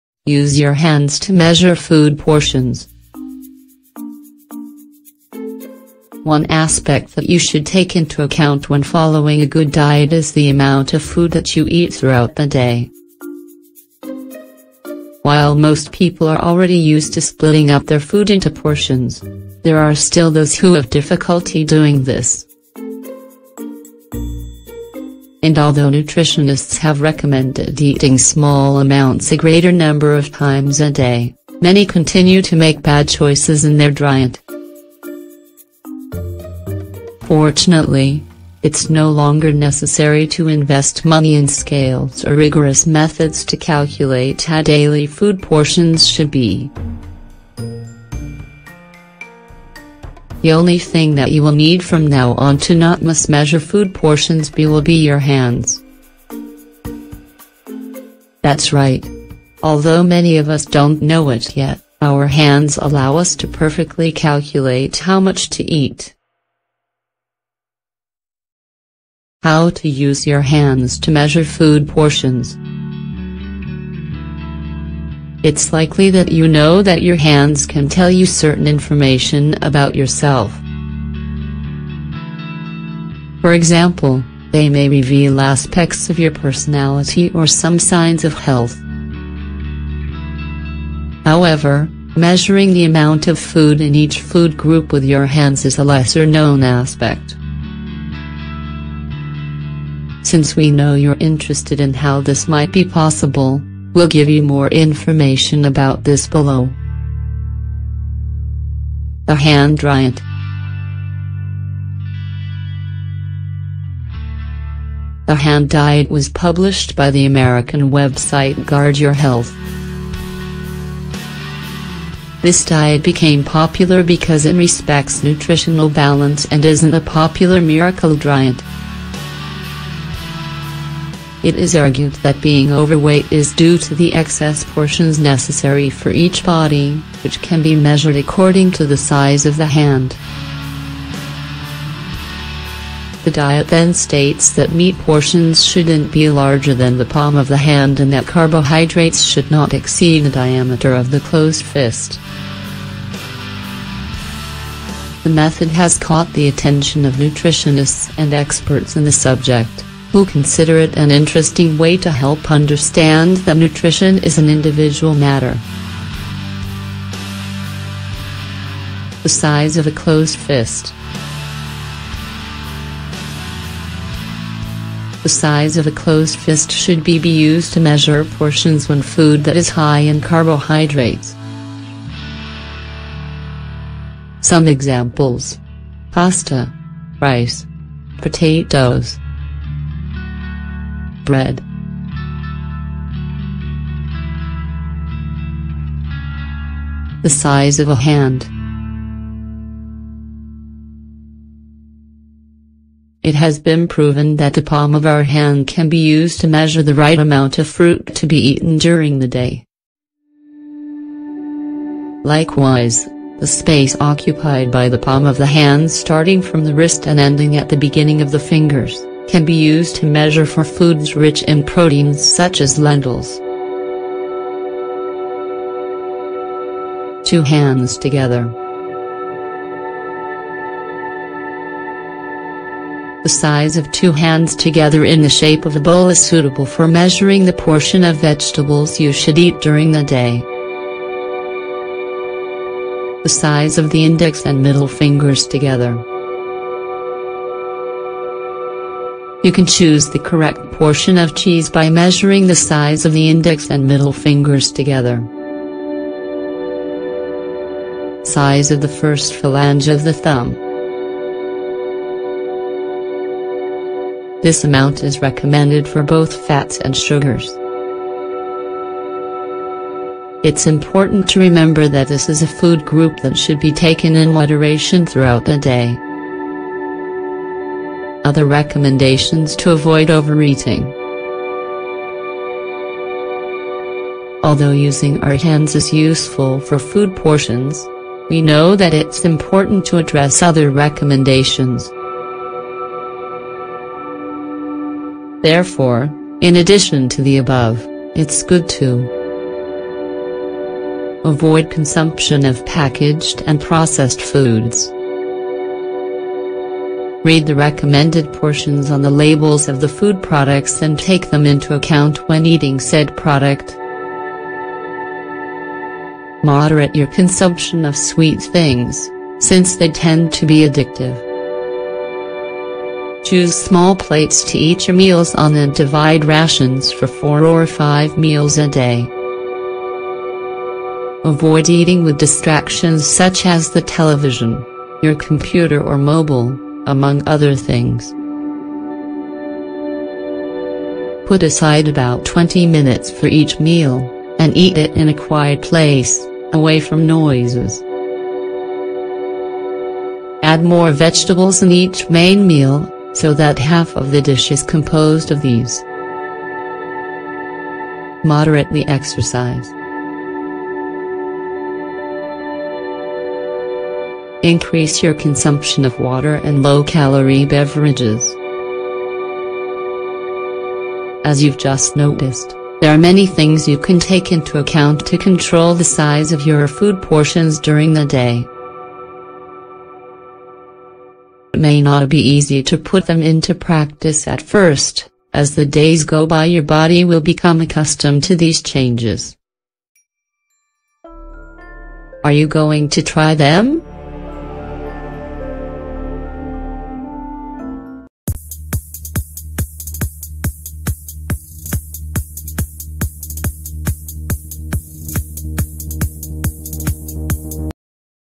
Use Your Hands to Measure Food Portions
Although we usually measure foods like rice with our hands, we can also use them to measure the food portions of many other types of food.